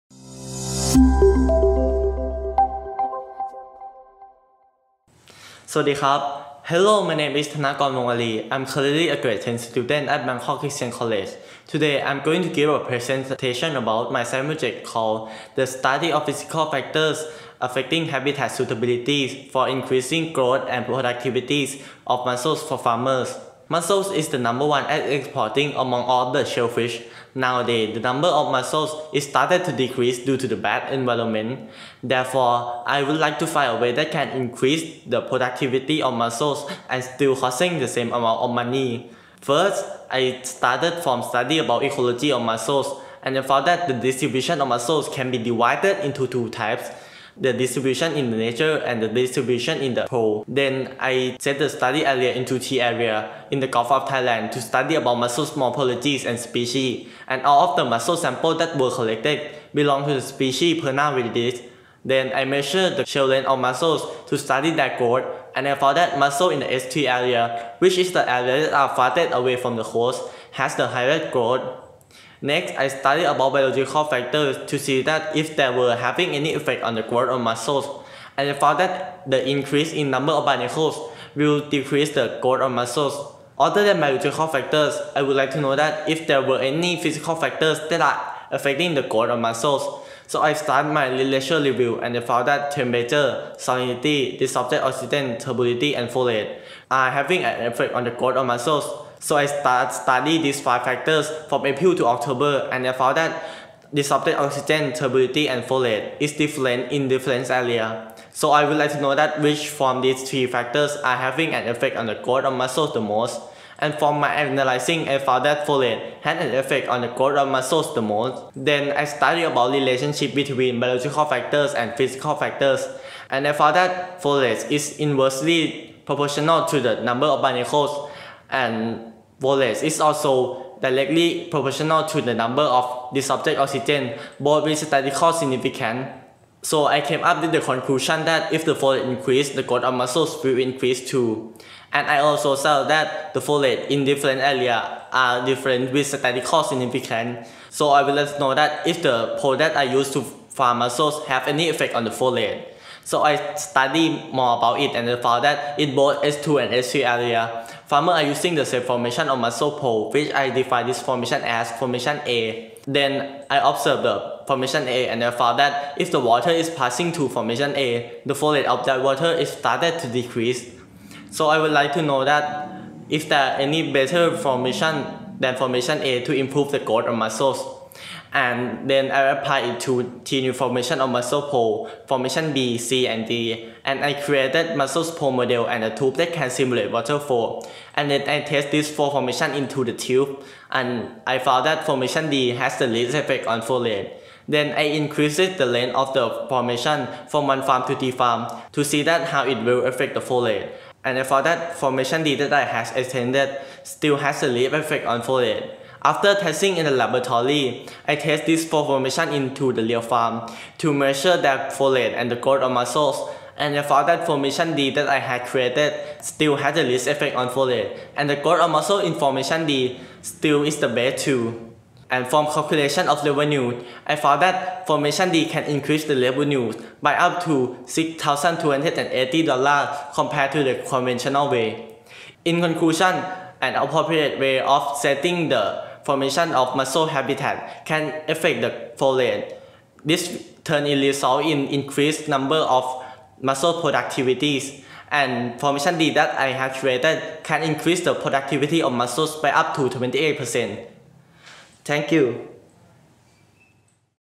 physical factors affecting habitats suitability for increasing growth and productivity of mussels fo
It is the last poster of APEC Online Conference for Teen Researcher.
Tanakorn Wongaree is the presenter of this research.